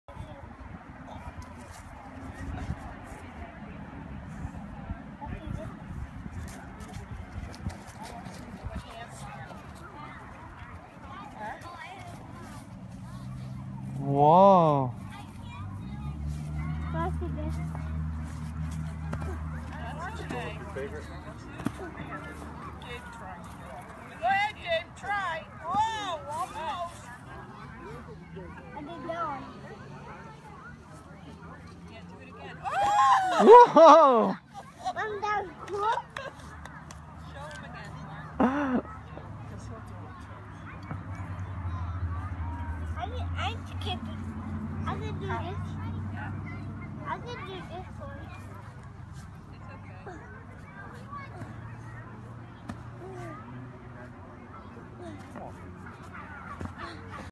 Gabe accidentally does Front Flip
Gabe does a front flip while playing around at Gozzi's Turkey Farm.